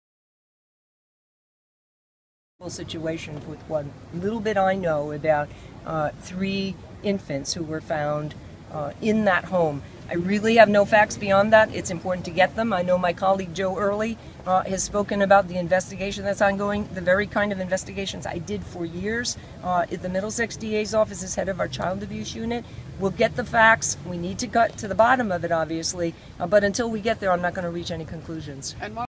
Video: Martha Coakley answers questions about the Blackstone house of horrors
Attorney General Martha Coakley answers questions from reporters on the developing situation in Blackstone where the bodies of three infants were pulled from a condemned home.